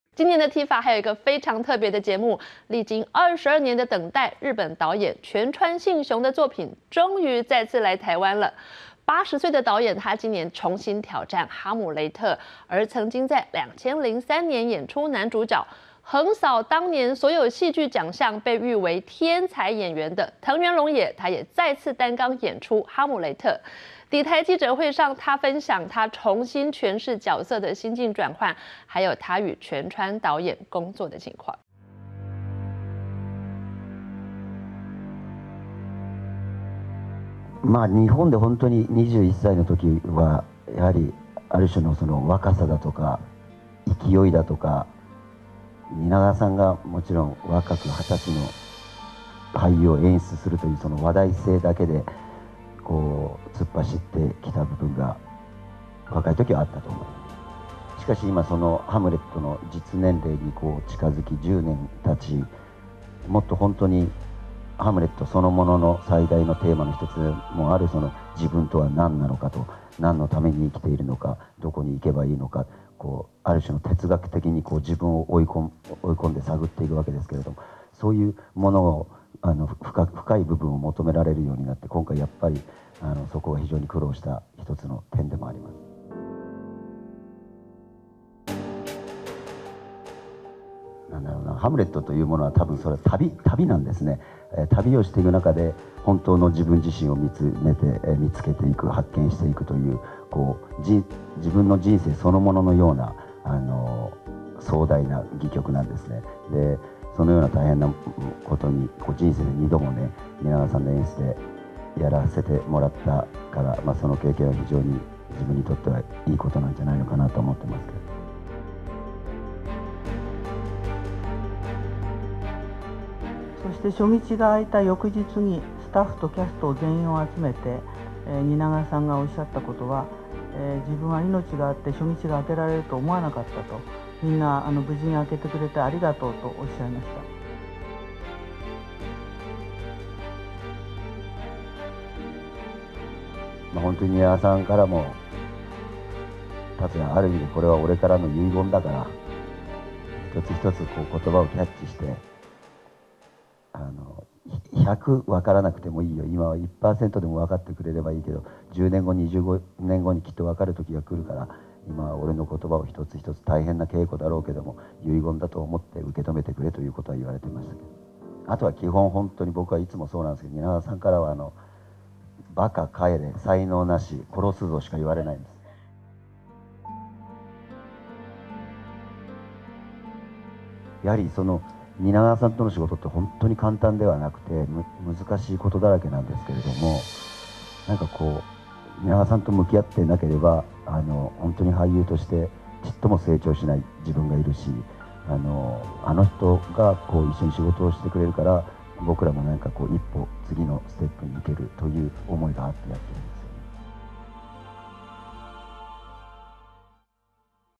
《藝想世界》睽違22年 蜷川幸雄《哈姆雷特》登台
日本導演蜷川幸雄的作品終於再度來台。80歲的蜷川幸雄重新挑戰《哈姆雷特》，而曾在2003年演出男主角，橫掃當年所有戲劇獎項，被譽為天才演員的藤原龍也，也再次擔綱演出哈姆雷特。抵台記者會上，藤原龍也分享重新詮釋角色的心境變換，以及與蜷川導演工作的情況。